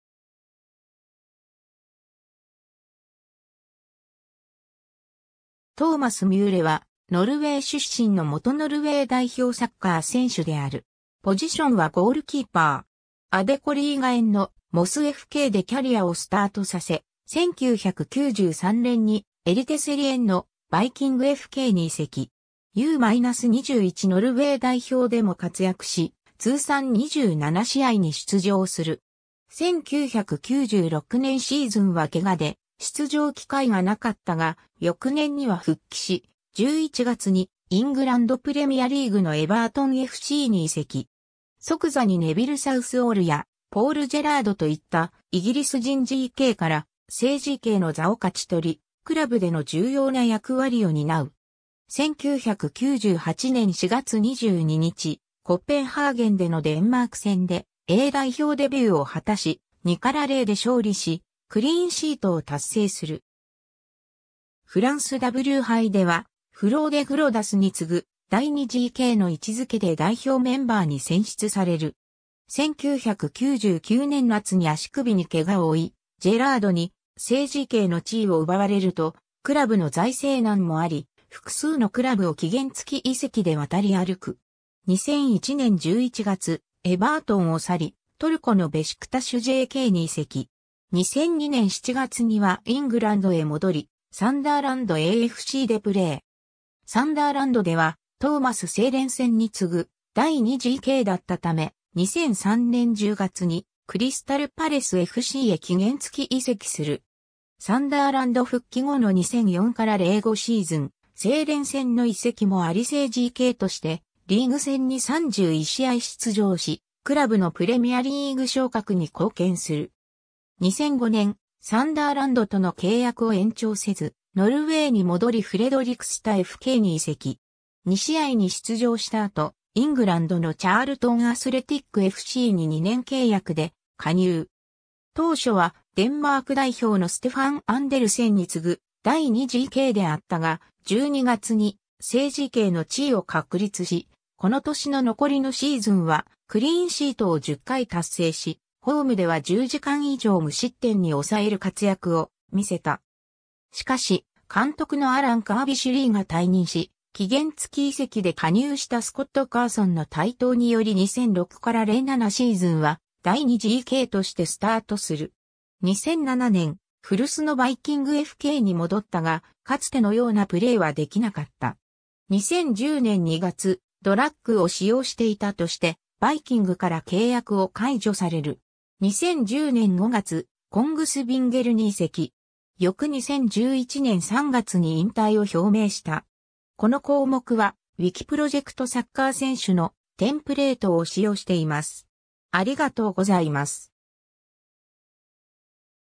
トーマス・ミューレ
トレンメア・ローヴァーズFCの選手
トーマス・ミューレ（Thomas Myhre, 1973年10月16日 - ）はノルウェー出身の元ノルウェー代表サッカー選手である。
 アデコリーガエン（2部）のモスFKでキャリアをスタートさせ、1993年にエリテセリエン（1部）のバイキングFKに移籍。
U-21ノルウェー代表でも活躍し、通算27試合に出場する。
1996年シーズンはケガで出場機会が無かったが、翌年には復帰し、11月にイングランド・プレミアリーグのエヴァートンFCに移籍。
即座にネヴィル・サウスオールやポール・ジェラードといったイギリス人GKから正GKの座を勝ち取り、クラブでの重要な役割を担う。
1998年4月22日、コペンハーゲンでのデンマーク戦でA代表デビューを果たし、2-0で勝利しクリーンシートを達成する。
フランスW杯ではフローデ・グロダスに次ぐ第2GKの位置付けで代表メンバーに選出される。
 1999年夏に足首にケガを負い、ジェラードに正GKの地位を奪われると、クラブの財政難もあり複数のクラブを期限付き移籍で渡り歩く。
2001年11月、エヴァートンを去りトルコのベシクタシュJKに移籍。
2002年7月にはイングランドへ戻りサンダーランドAFCでプレー。
サンダーランドではトーマス・セーレンセンに次ぐ第2GKだったため、2003年10月にクリスタル・パレスFCへ期限付き移籍する。
サンダーランド復帰後の2004-05シーズン、セーレンセンの移籍もあり正GKとしてリーグ戦に31試合出場し、クラブのプレミアリーグ昇格に貢献する。
 2005年、サンダーランドとの契約を延長せず、ノルウェーに戻りフレドリクスタFKに移籍。
2試合に出場した後、イングランドのチャールトン・アスレティックFCに2年契約で加入。
当初はデンマーク代表のステファン・アンデルセンに次ぐ第2GKであったが、12月に正GKの地位を確立し、この年の残りのシーズンはクリーンシートを10回達成し、ホームでは10時間以上無失点に抑える活躍を見せた。
しかし、監督のアラン・カービシュリーが退任し、期限付き移籍で加入したスコット・カーソンの台頭により2006-07シーズンは第2GKとしてスタートする。
 2007年、古巣のバイキングFKに戻ったが、かつての様なプレーは出来なかった。
2010年2月、ドラッグを使用していたとしてバイキングから契約を解除される。
 2010年5月、コングスビンゲルに移籍。
翌2011年3月に引退を表明した。
 この項目は、ウィキプロジェクト サッカー選手の「テンプレート」を使用しています。